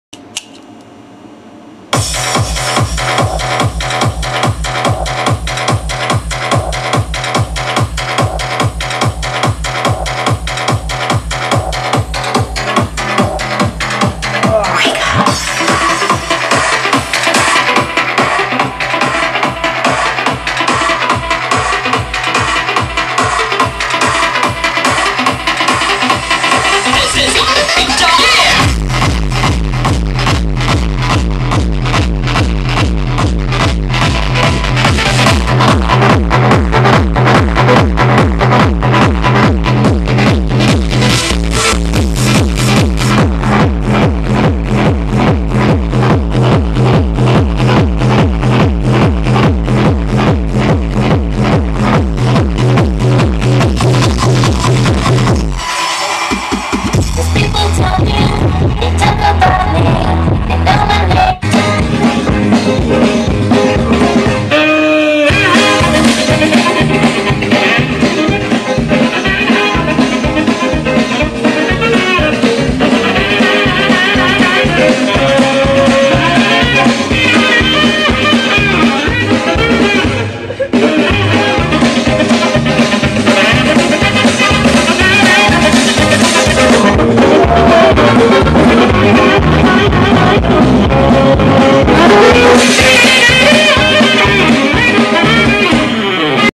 Cerwin vega clsc215 and clsc15!
Playing some cool music with my towers :D